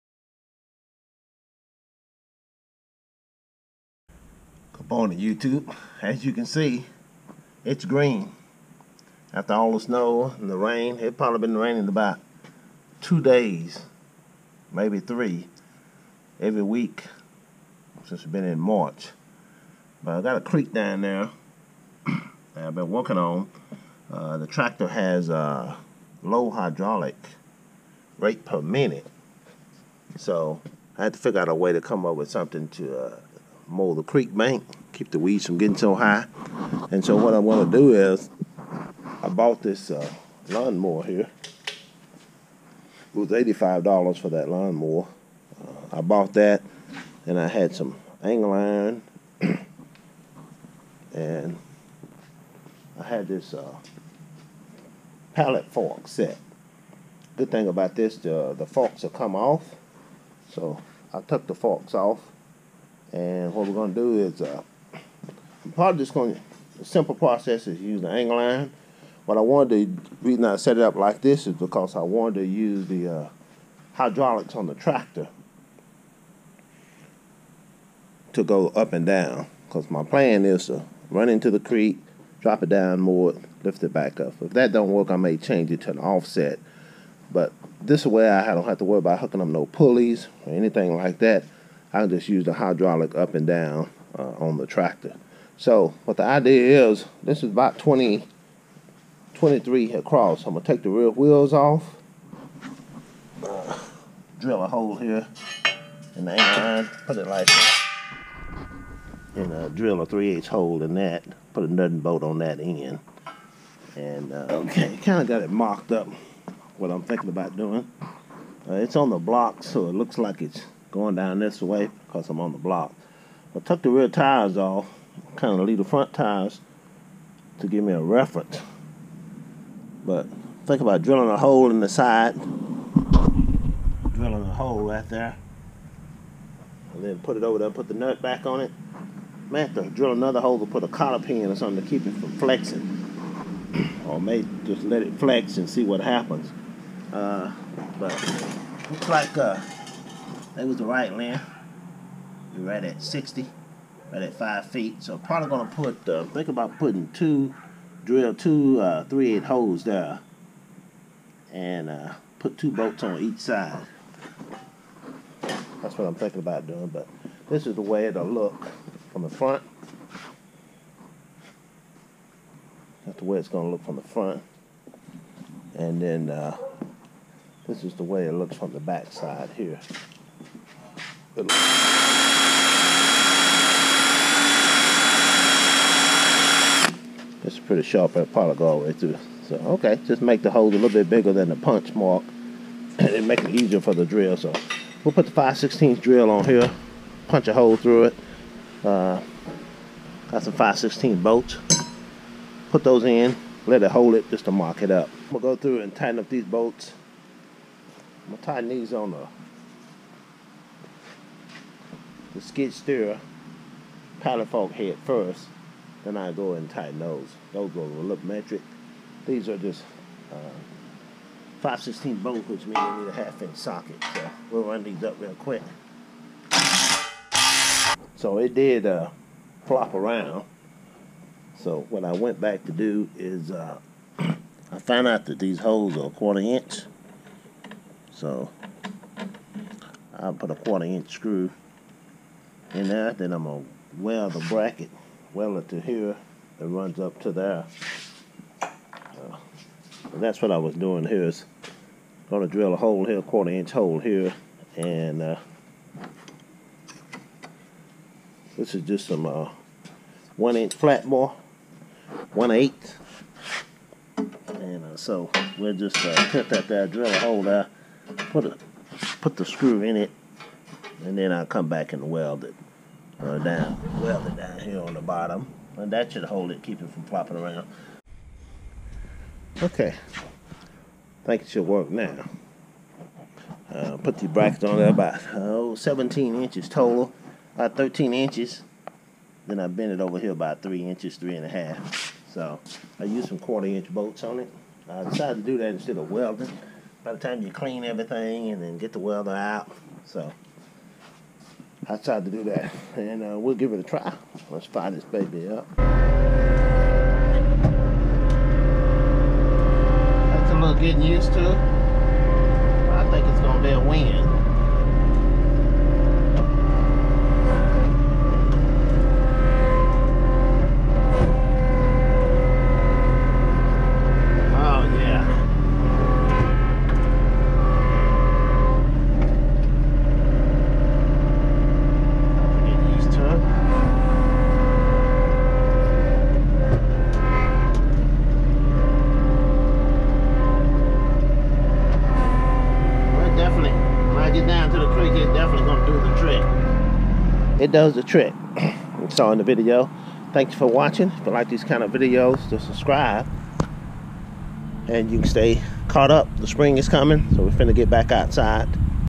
DIY Bank Mower Mod
I have a creek on my property and I wanted a way to easily cut the grass.   I purchased a push mower and used some angle iron to attach it to my tractor.  The grass is growing fast due to the rain we have received recently.